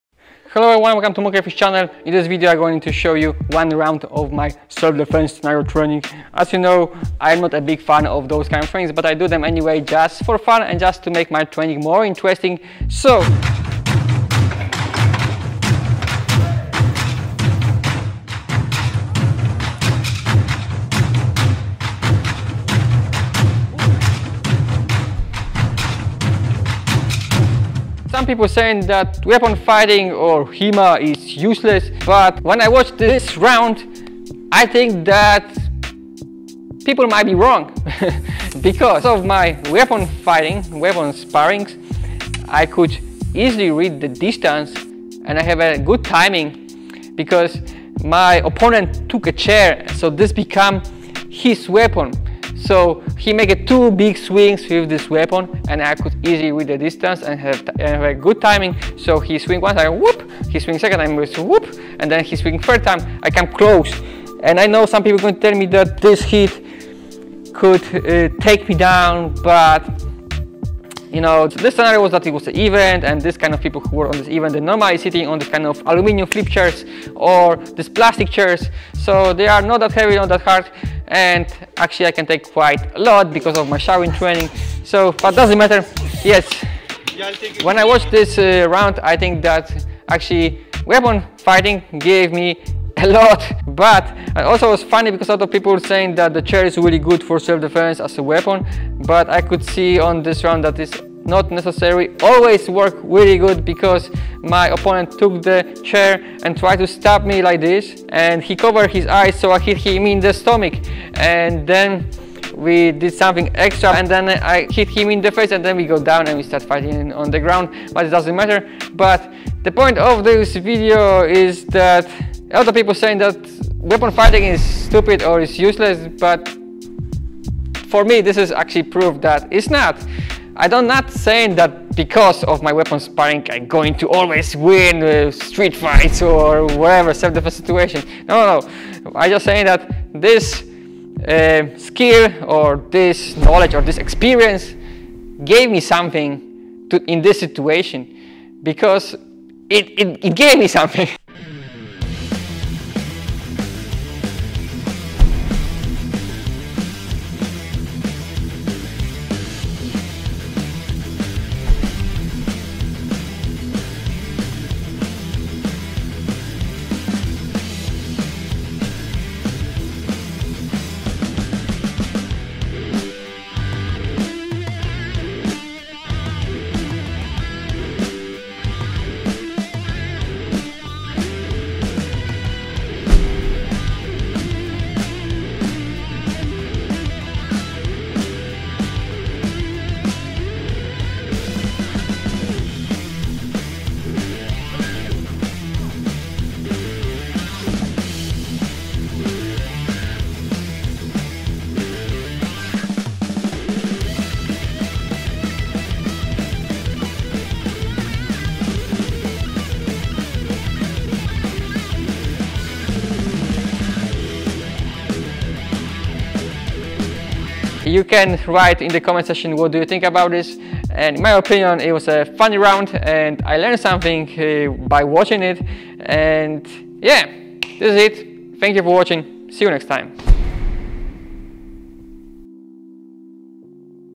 Is weapon training stupid?
Some people like to say that weapon training is useless because no one ouedays is carry swords.
I have a different opinion...